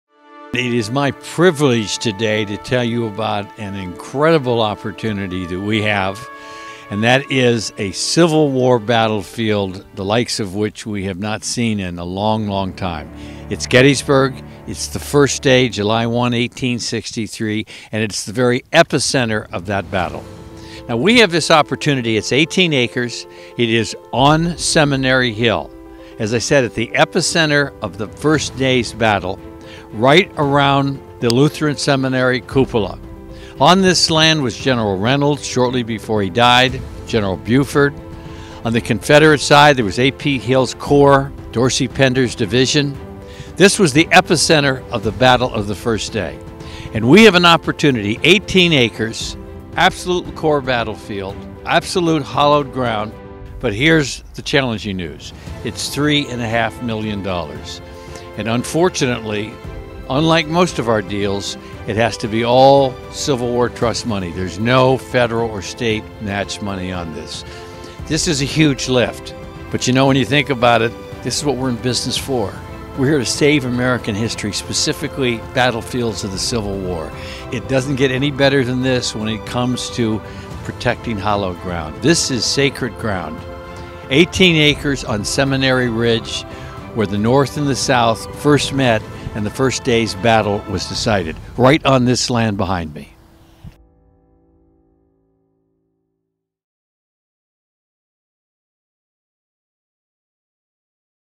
Help Save Seminary Ridge at Gettysburg
As of February 25, 2019 this land has officially been saved. Thanks to everyone that donated or showed interest!

We are excited to announce an amazing opportunity to save the land on Seminary Ridge where the Theological Seminary sits. This is where Union generals John Reynolds and John Buford first clashed with Confederates A.P. Hill and Dorsey Pender on the first day at Gettysburg. Click here to be a part of this first preservation opportunity under our new umbrella name, the American Battlefield Trust.